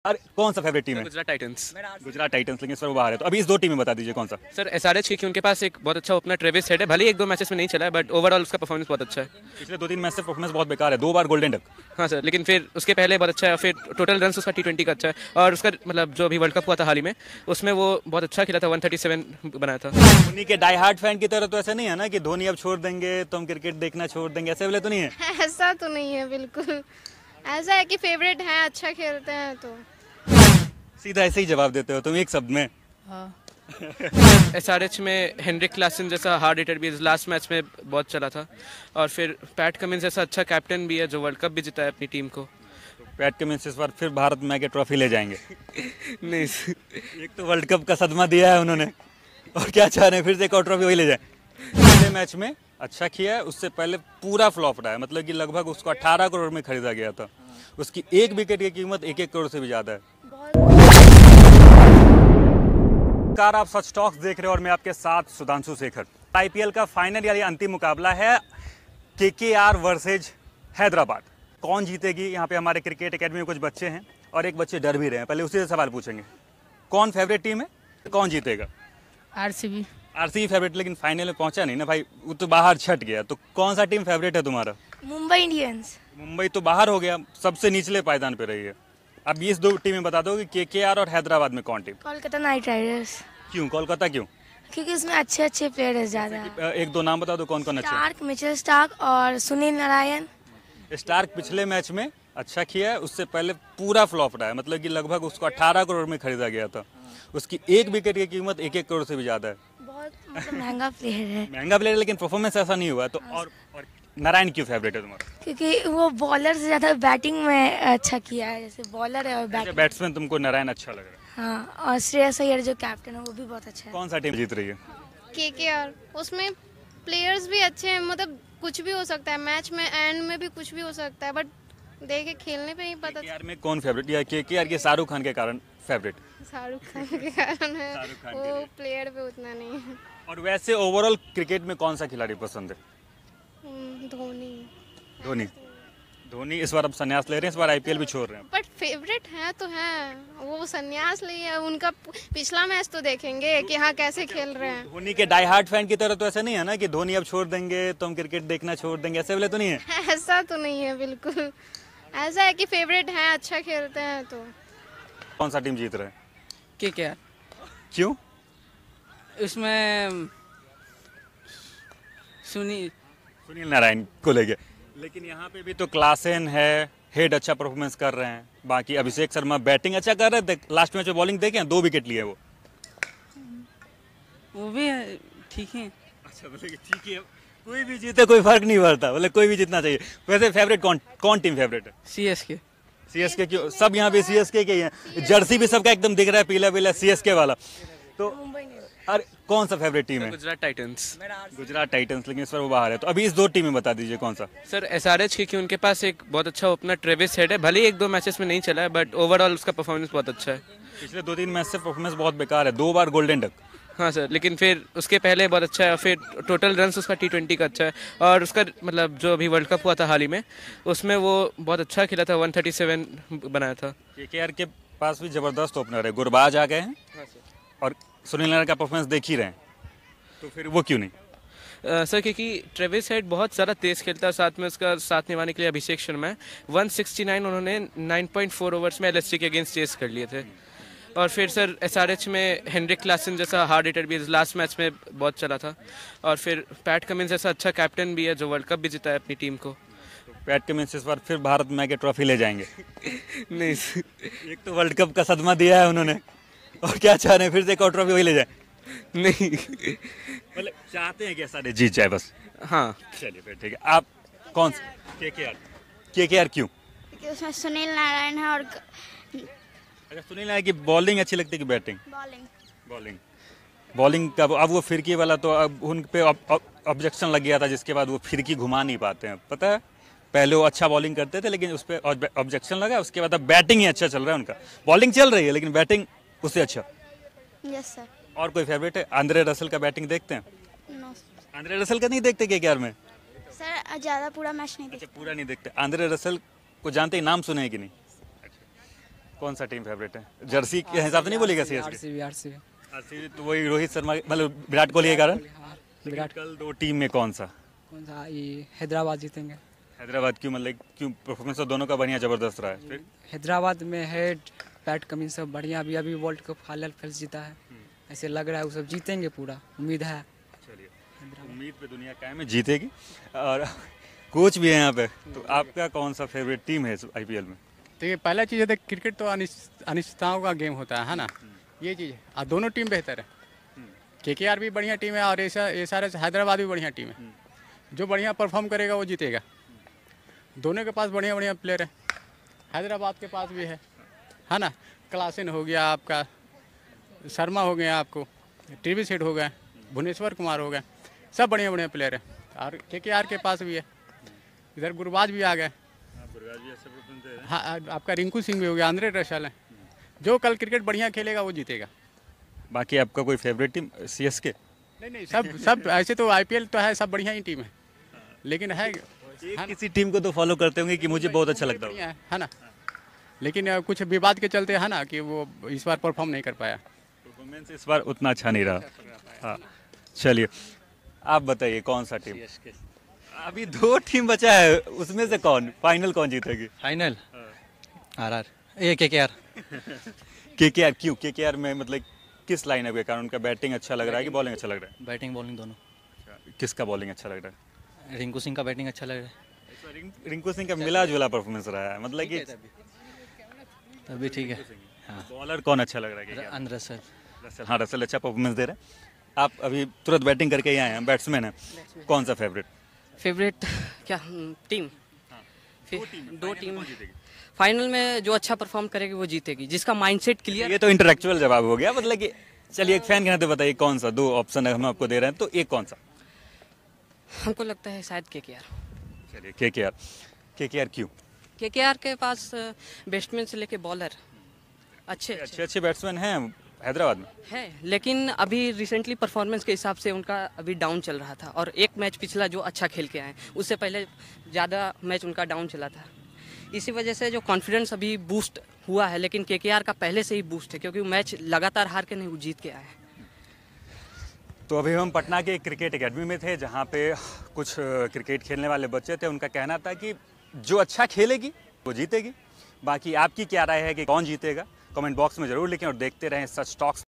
KKR VS SRH: कौन जीतेगा IPL 2024 का फाइनल ? बिहार के युवा क्रिकेटर किसे मान रहे दावेदार ?
KKR VS SRH: कौन जीतेगा IPL 2024 का फाइनल ? बिहार के युवा क्रिकेट खिलाड़ी किस मान रहे दावेदार ?